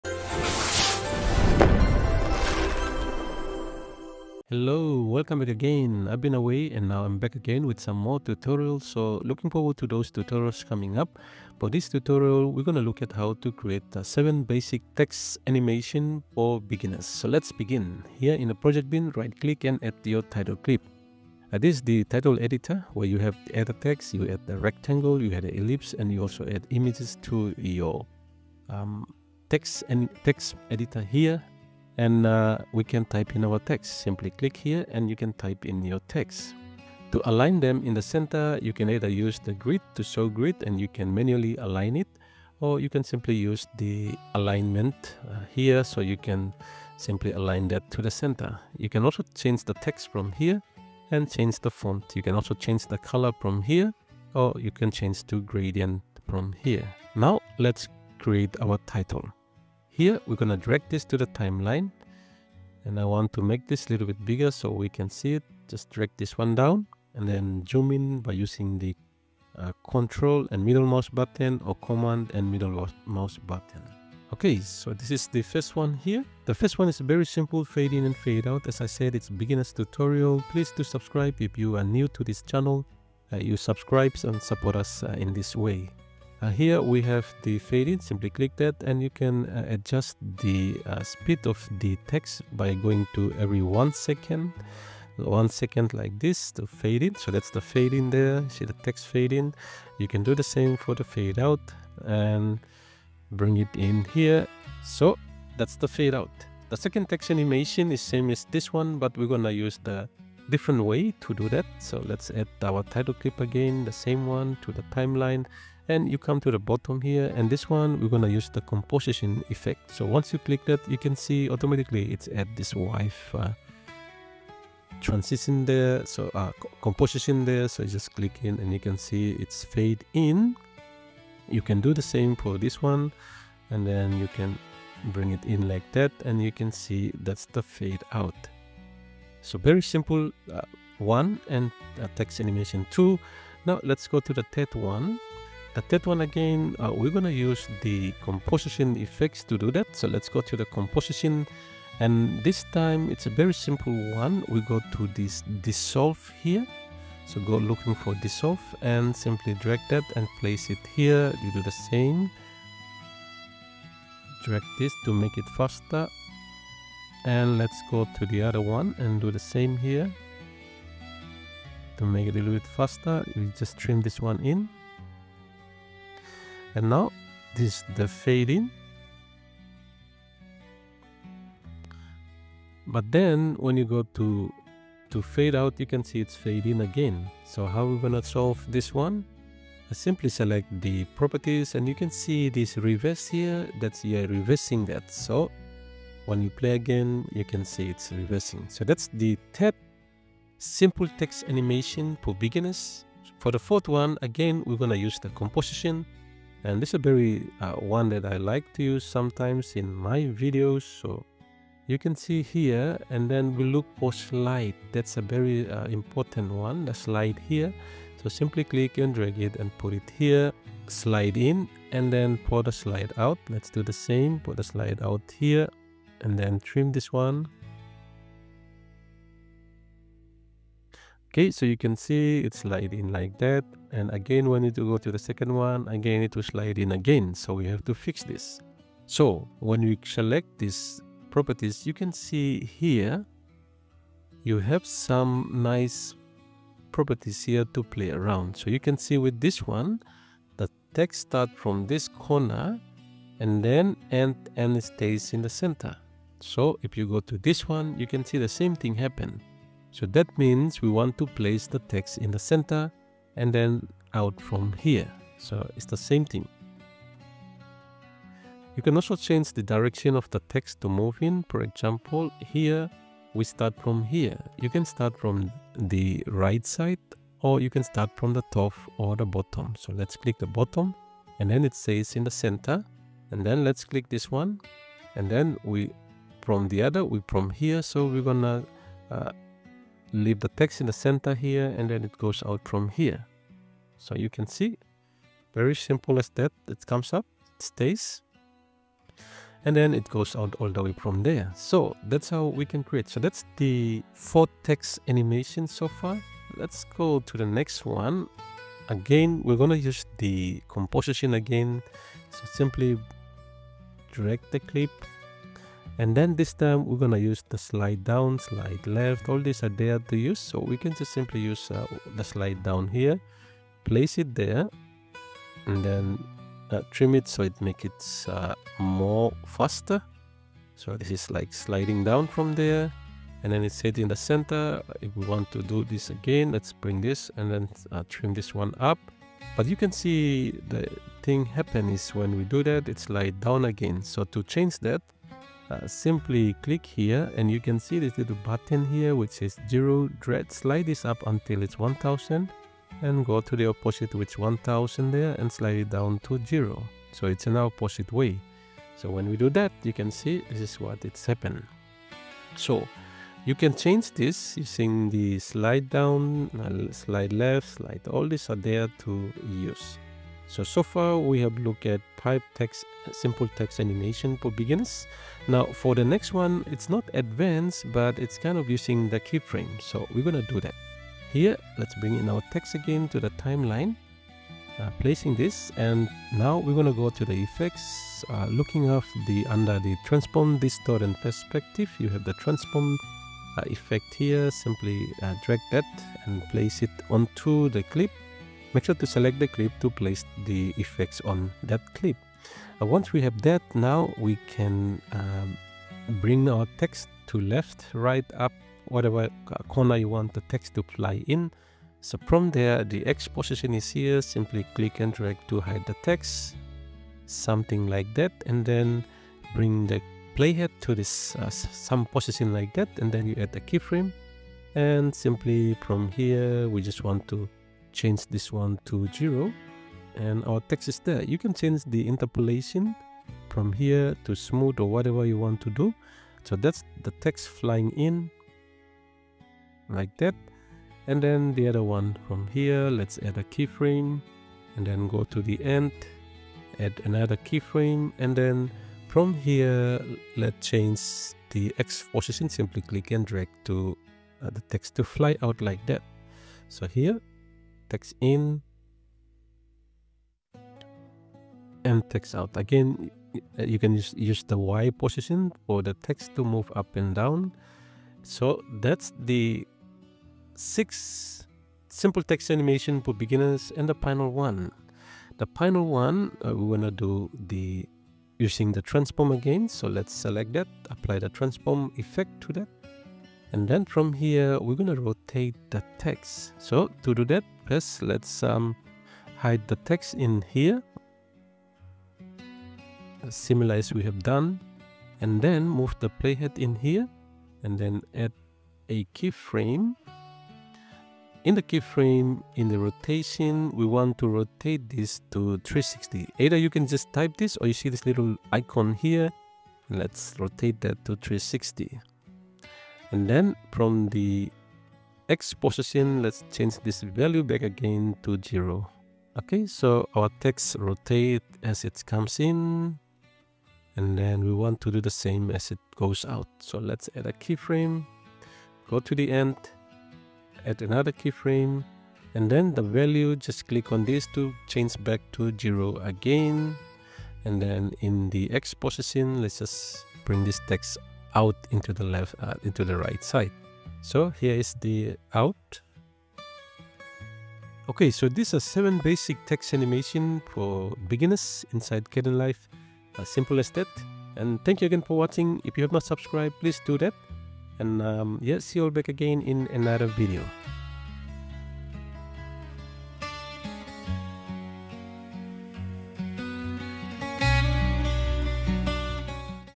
Kdenlive: 7 Simple Text Animation Tutorials for Beginners
Welcome to Eses Infor Learning, your go-to destination for immersive and educational content in the realm of free video editors.

In Video we look at 7 beginners text animation tutorials. Hope this helps you.

Timestamp